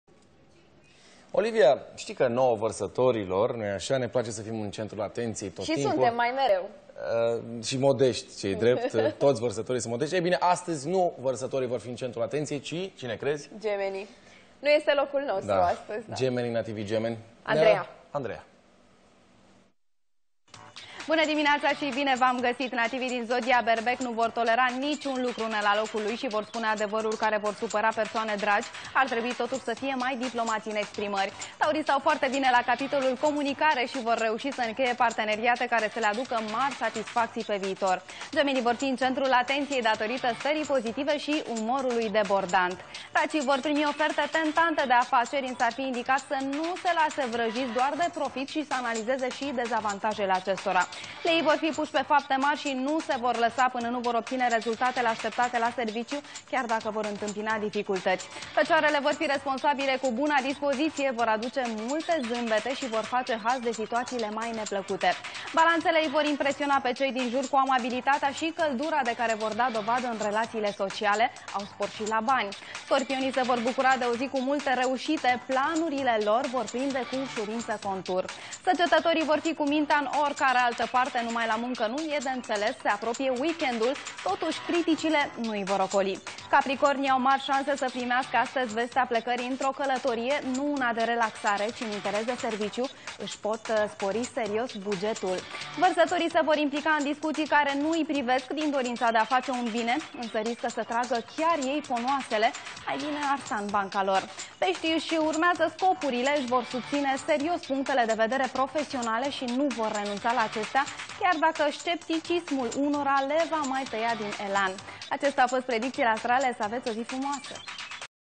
Horoscopul zilei 5/09/2014
Berbecii nu vor tolera niciun lucru nelalocul lui si vor spune adevaruri care vor supara persoane dragi. Ar trebui totusi sa fie mai diplomati in exprimari., Stiri, Antena 1, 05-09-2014